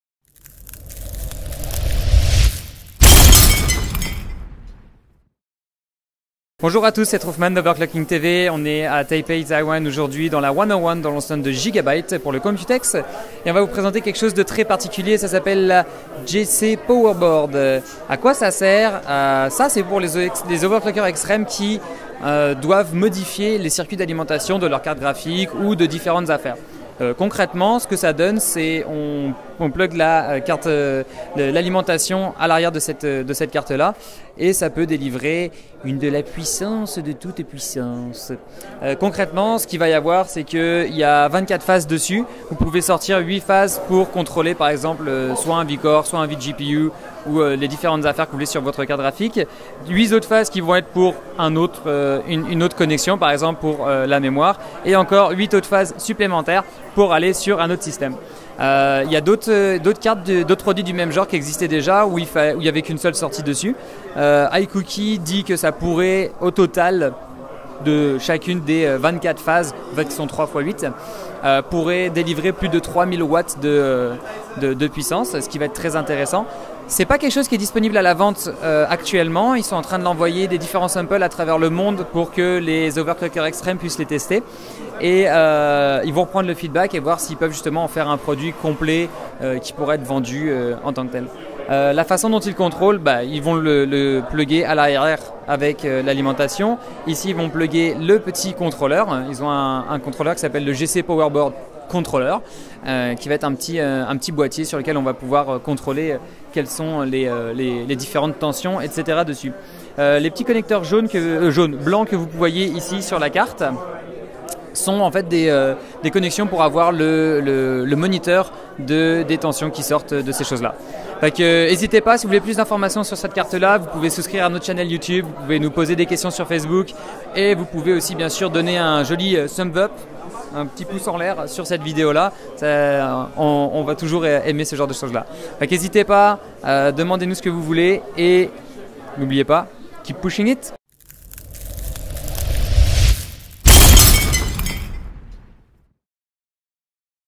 Présentation de la Gigabyte G-Powerboard au Computex 2014
Ceci est une carte qui développée par Gigabyte pour permettre aux overclockers en besoin de puissance d'ajouter facilement des phases d'alimentation supplémentaires à leur system.